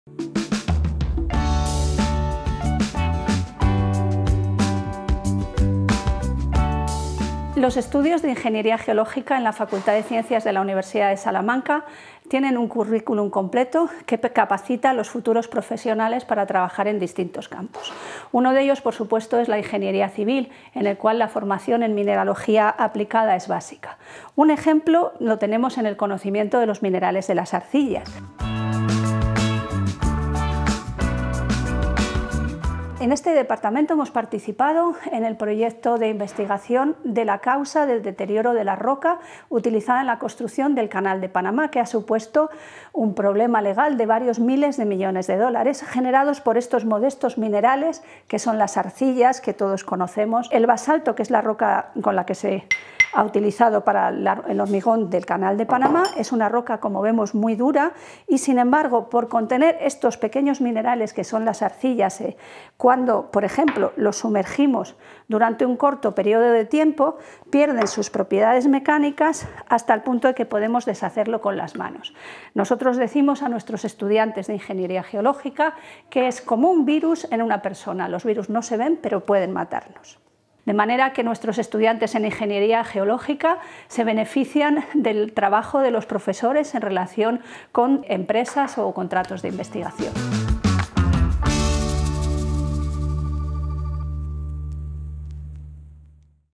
Grado en Ingeniería Geológica de la Universidad de Salamanca
Estudia el Grado en Ingeniería Geológica de la Universidad de Salamanca y accede a campos profesionales tan diversos como la Ingeniería Civil.
El Departamento de Geología de la USAL ha participado recientemente en iniciativas como el proyecto de investigación de la causa del deterioro de la roca utilizada en la construcción del Canal de Panamá.